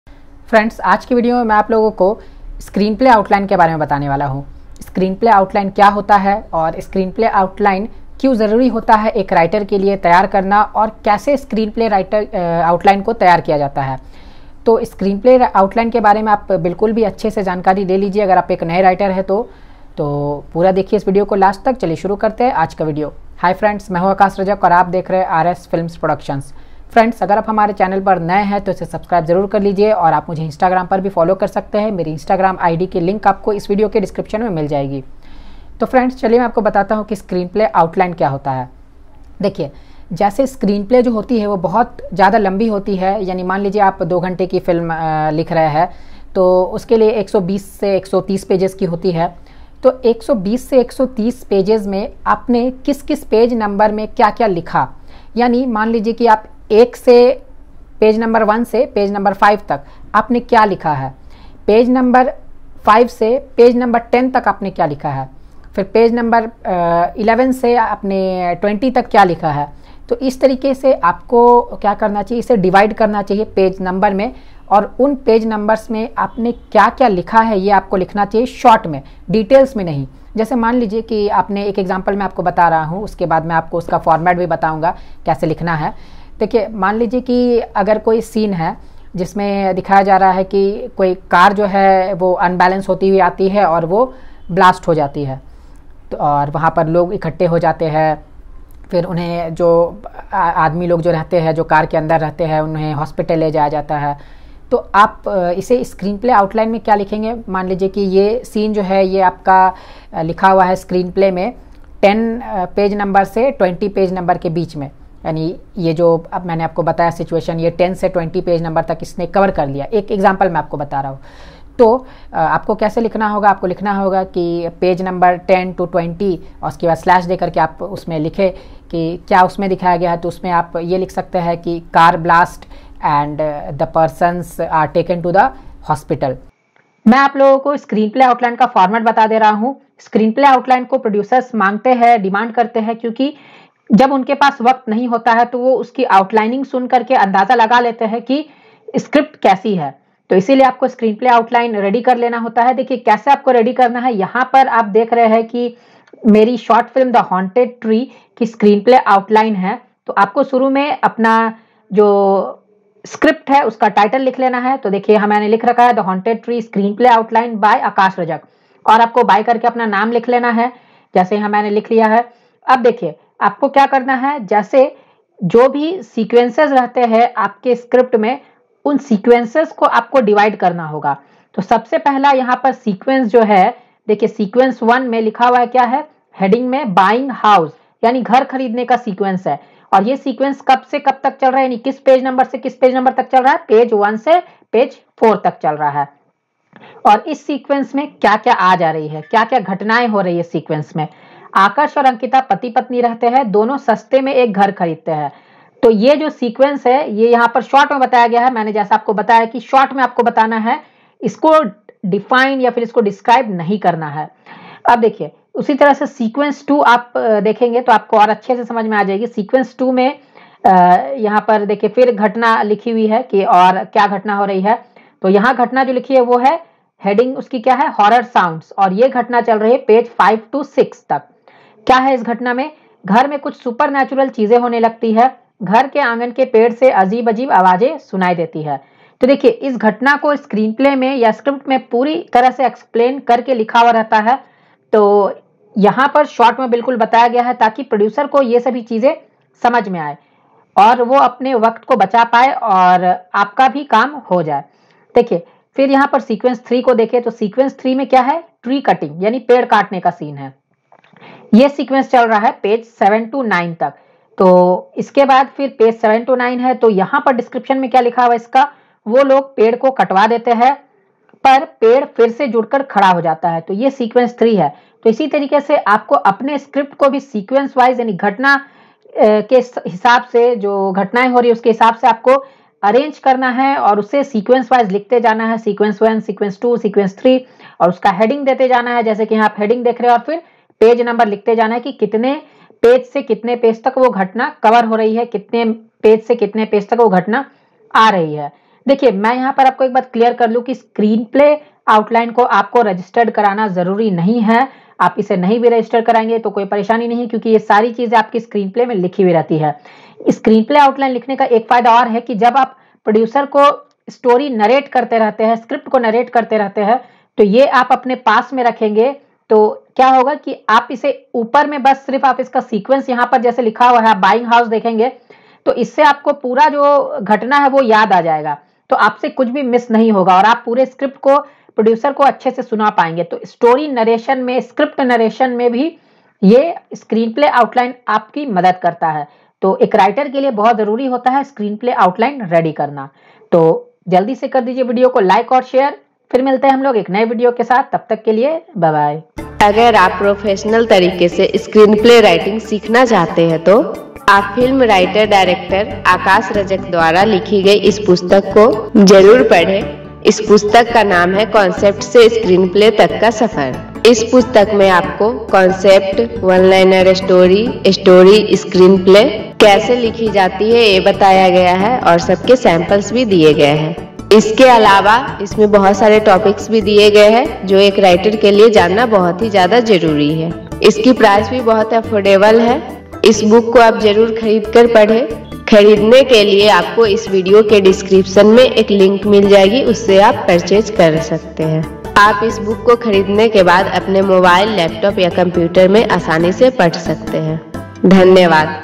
What is outline in screenplay | How to write a script outline | Script writing tips for beginners
Meri  screenplay writing ki ebook ko kharidne ke liye niche diye  gaye link per click kare :

This video is about what is outline in screenplay. How to write a script outline. I have given script writing tips for beginners.

Thankyou.